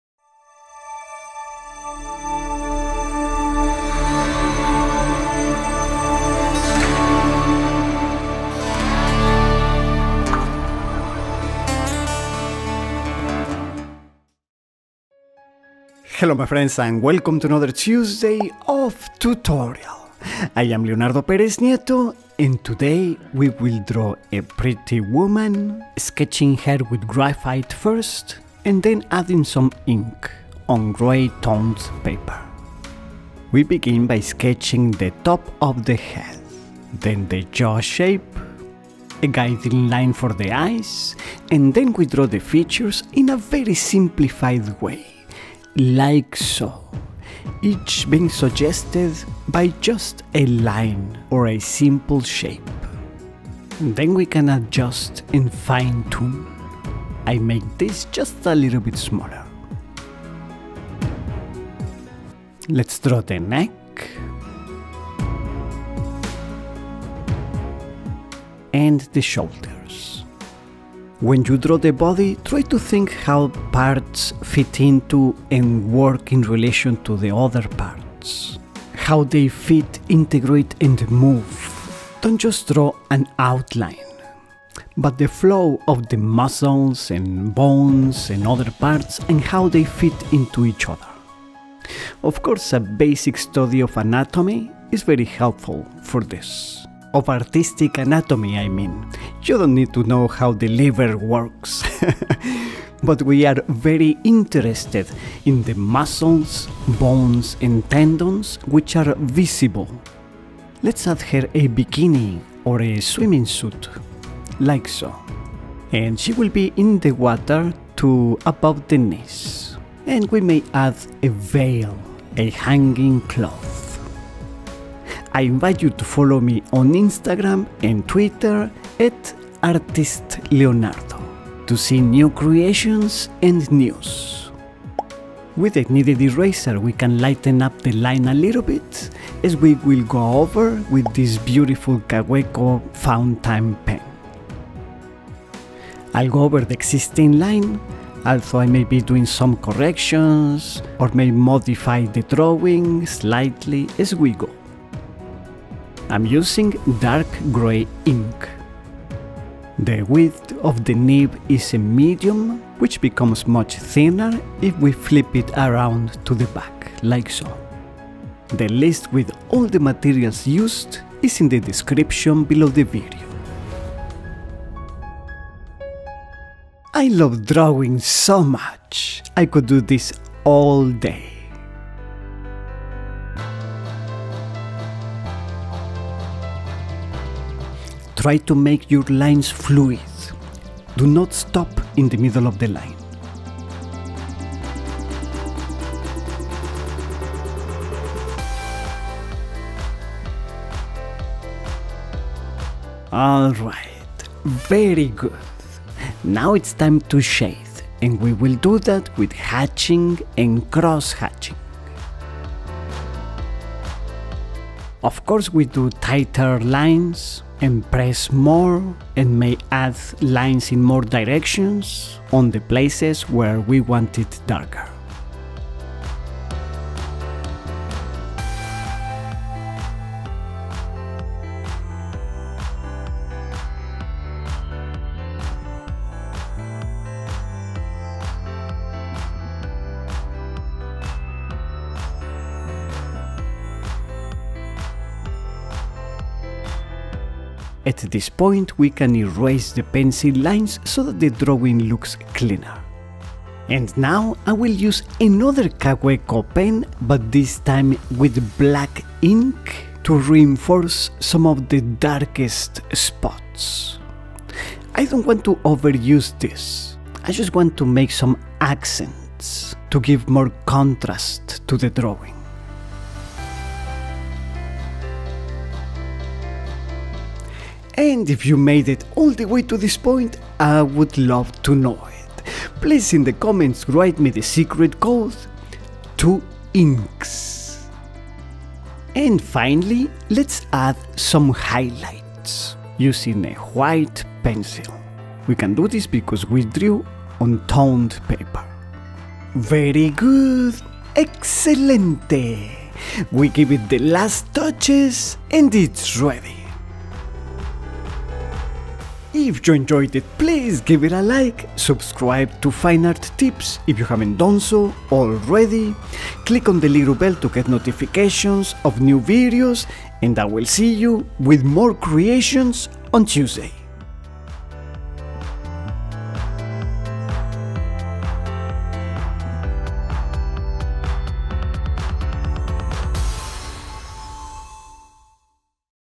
HOW TO DRAW A PRETTY WOMAN IN A BIKINI - pen & ink - Fountain Pen
This video shows how to draw a pretty woman in a bikini using a fountain pen.

List of materials:

- Mechanical pencil: Tutto3, with 0.3 mm 2H lead, 0.5 mm HB lead, and 0.7 mm 2B lead:
Tutto3 Black
Tutto3 Silver

- Kaweco Sport Steel fountain pen, M nib. (the one in the link has a fine nib, which is excellent for drawing too)

- Kaweco grey ink cartridges

- Kaweco Skyline Sport Fountain Pen Black M

- Kaweco black ink cartridges

- White : Primo Bianco Charcoal 59-W - See it

- Strathmore toned gray paper 9” x 12” (22.9 x 30.5 cm)

(To record the voice-over) Mic: Blue Yeti